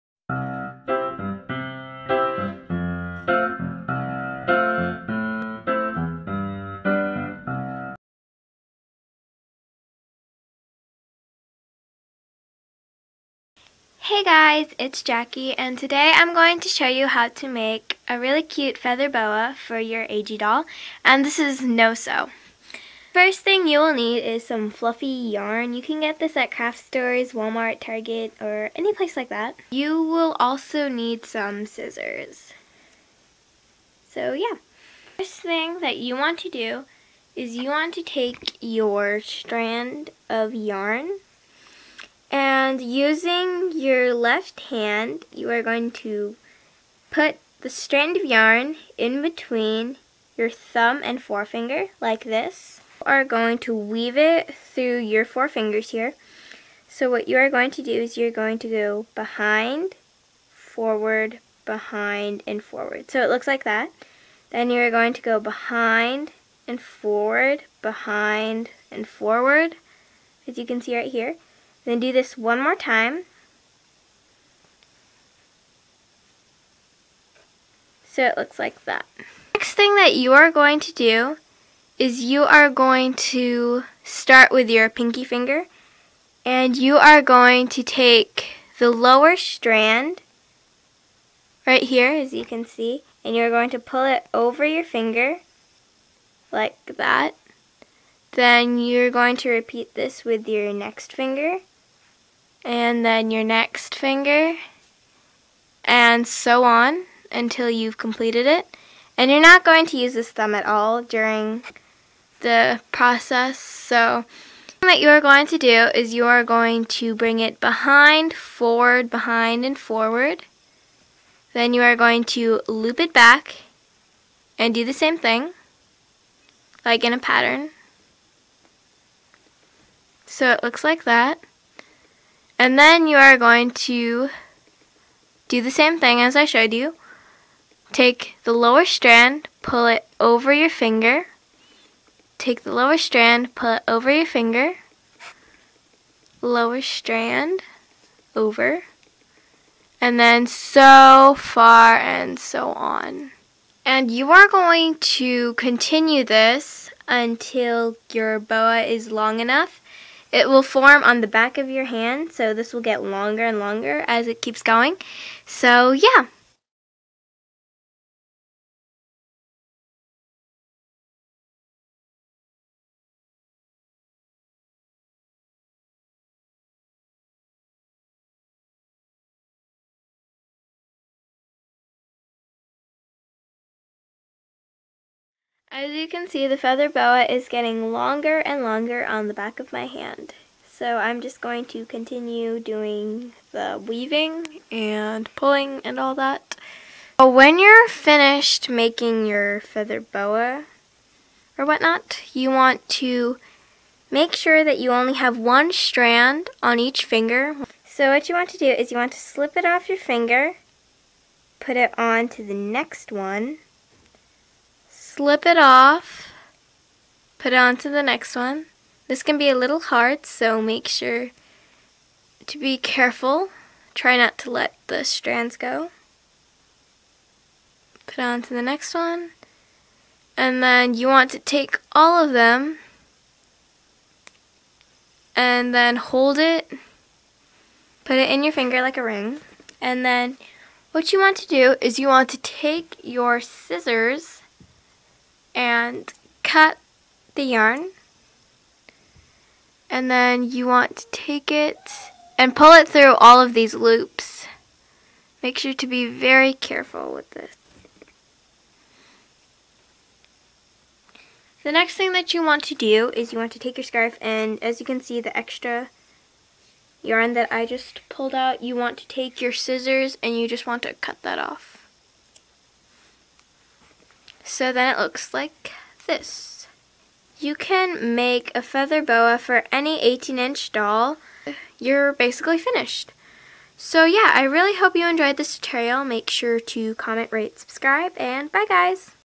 Mini Craft Show ~ Episode 3: Feather Boa/Scarf for Dolls
I saw this a long time ago on AG's website and added fluffy yarn to make a feather boa. :)

This is my video for the week, I hope you enjoy, I took so many takes on this. XD